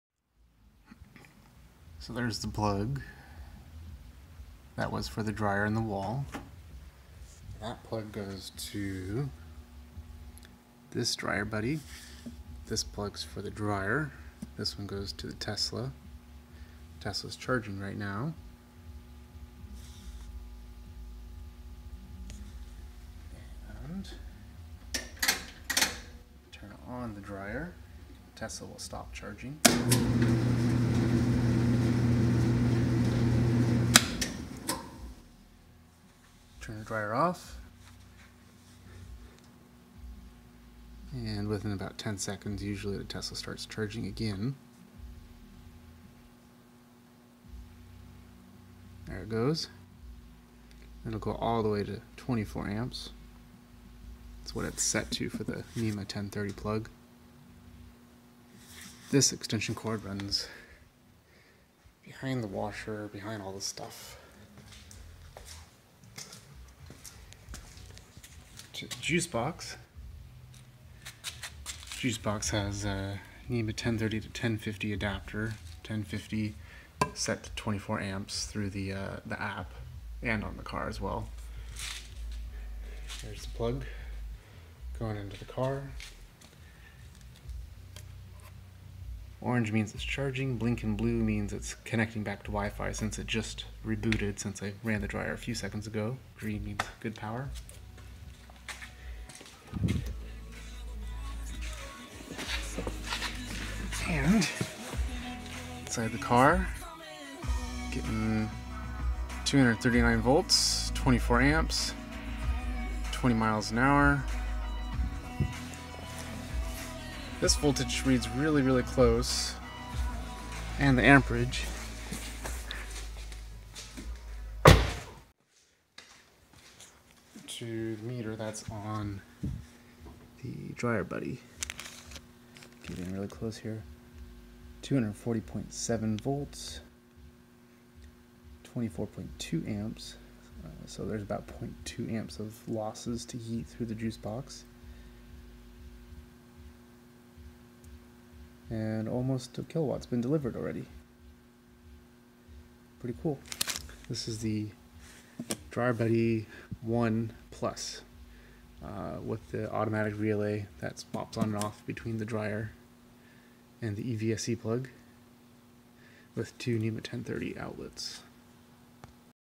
Sharing a 10-30 dryer outlet with Telsa Model 3 and dryer with Dryer Buddy #1 Plus Auto
I am renting a house and have one NEMA 10-30 dryer outlet, the Dryer Buddy #1 plus auto ended up being the best solution to share the one outlet between the electric dryer and the Tesla model 3 without any switches to flip or risk of overloading any circuits.

The electrical panel on this house is full and would have been extremely expensive to upgrade my sub panel in my rental. This works with the regular supplied model 3 UMC charger as well, I just have an extra Juice Box I use this with.

Items in this video:

Dryer Buddy is sold by BSA Electronics and is avalible here for $306.39 as of 4/30/2018

25' Nema 10-30 30 Amp Extension cord

JuiceBox Pro 40 EVSE Charger with Wifi and Juice net